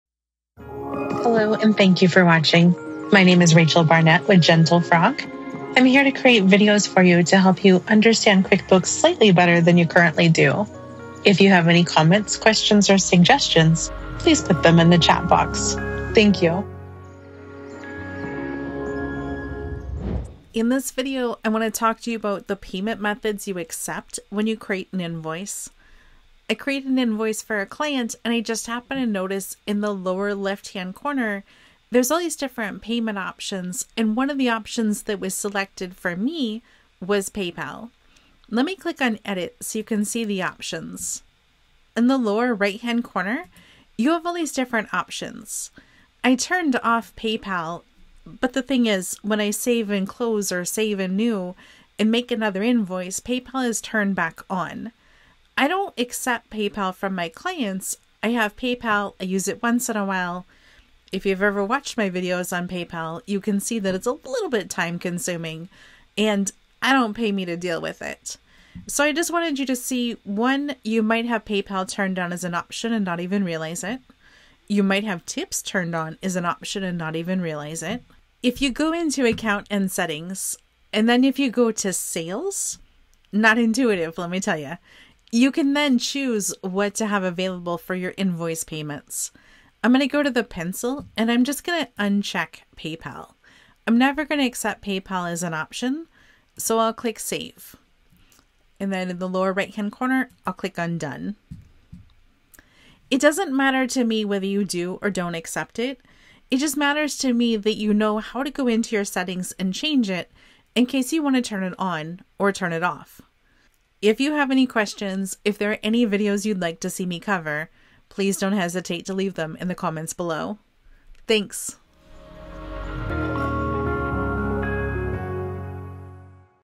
How to Customize Payment Methods for Invoices in QuickBooks Online
Navigating payment method options for your invoices in QuickBooks Online can impact your invoicing process and your interaction with clients. In this quick tutorial, you'll learn how you can customize the payment methods you accept when creating an invoice in QuickBooks Online. Specifically, I noticed that PayPal was automatically selected as a payment option on my invoices, which might not suit everyone's business needs, including mine.

What You'll Learn:
- Discovering Payment Options: How to find and review the payment method options available on your QuickBooks Online invoices.
- Editing Payment Methods: Editing and customizing which payment methods you'd like to offer to your clients on the invoice.
- Editing Payment Methods in Account Settings: Instructions on how to access your account and settings to adjust your invoice payment options effectively.
- Turning PayPal On/Off: How to specifically enable or disable PayPal as a payment method for your invoices, depending on your business preferences.

 It’s crucial that your invoicing process aligns with your business operations and payment preferences. This video will show you how to make adjustments to your payment options within your QuickBooks Online settings.

This tutorial is perfect for small business owners, freelancers, and anyone using QuickBooks Online to manage their finances and looking to streamline their invoicing process by customizing their payment method options.

Timestamps
00:00 - Intro
00:28 - Customer payment options
01:17 - Edit Payment options in Account and Settings
01:50 - Outro